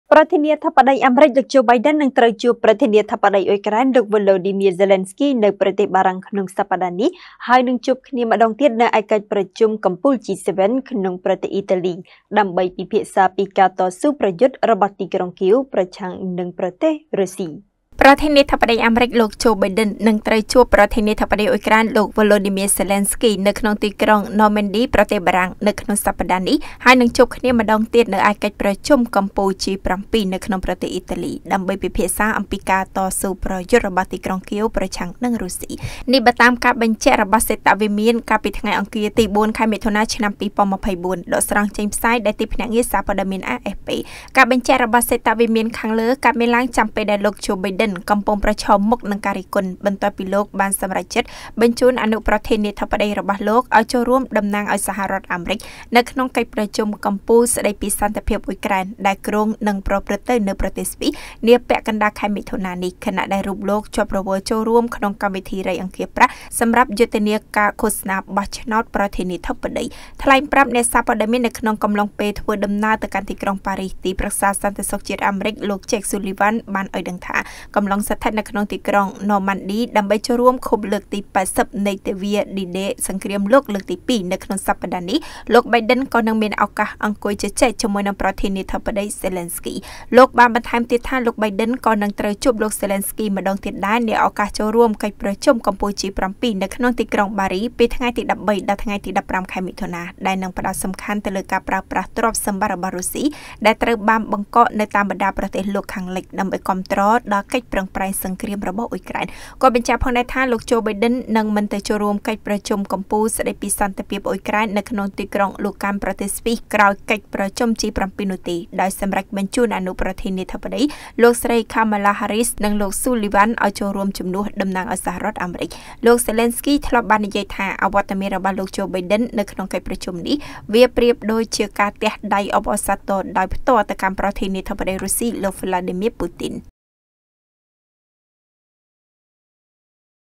លោក ចូ បៃដិន នឹងជួបលោក ហ្សេលេនស្គី នៅបារាំង និងកិច្ចប្រជុំ G7 នៅអ៉ីតាលី.
* រាយការណ៍ដោយ កញ្ញា សំ ស្រីនីត​ និងកញ្ញា​ យ៉ង់ សក្គុណា